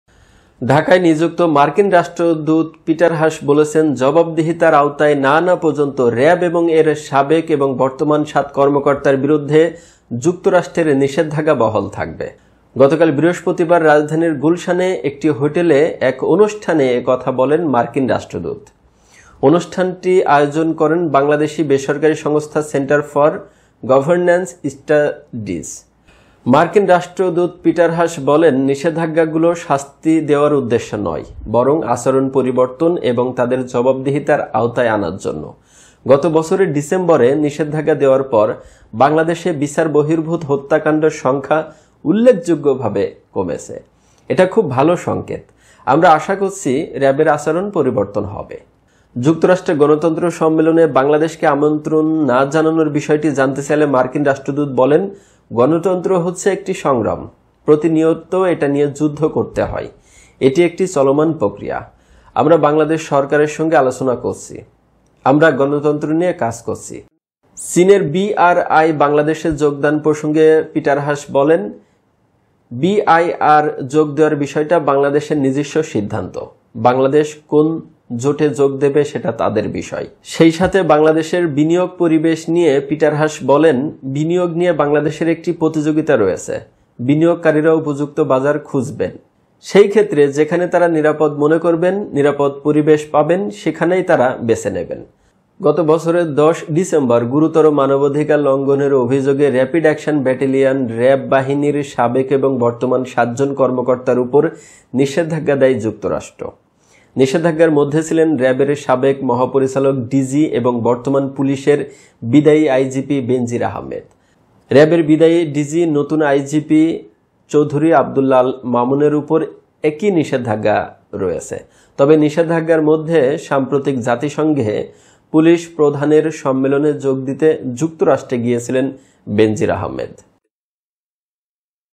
জবাবদিহির আওতায় না আনা পর্যন্ত র‍্যাবের ওপর নিষেধাজ্ঞা থাকবে !
ঢাকায় নিযুক্ত মার্কিন রাষ্ট্রদূত পিটার হাস বলেছেন, ‘জবাবদিহির আওতায় না আনা পর্যন্ত র‌্যাব এবং এর সাবেক ও বর্তমান সাত কর্মকর্তার বিরুদ্ধে যুক্তরাষ্ট্রের নিষেধাজ্ঞা বহাল থাকবে।